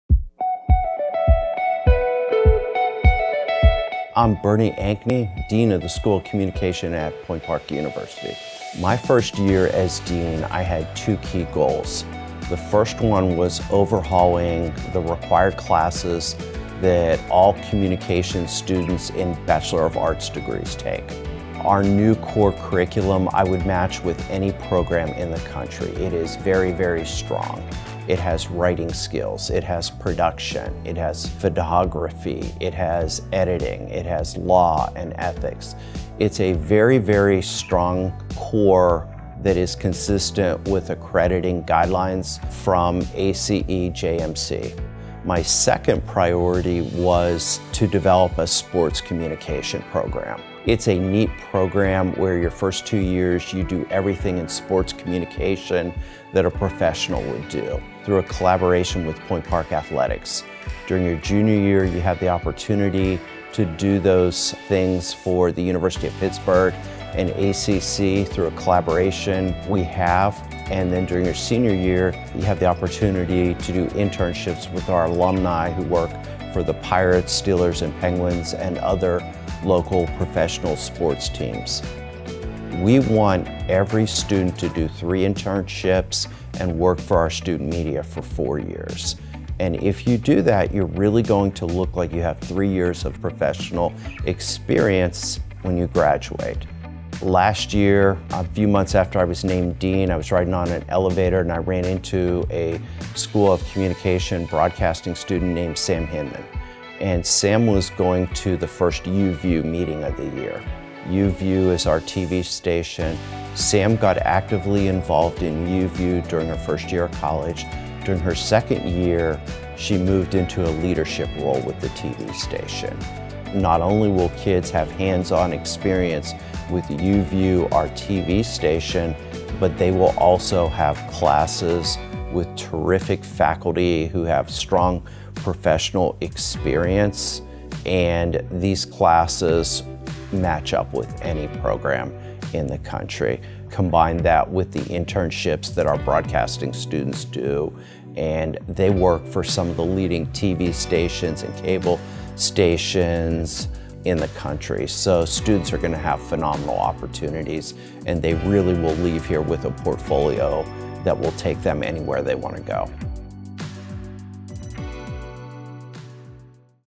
Dean Bernie Ankney on Point Park's School of Communication
From journalism to broadcasting and photography to sports communication, hear from Dean Bernie Ankney as he describes how Point Park's School of Communication makes its students career-ready in the field of their choice.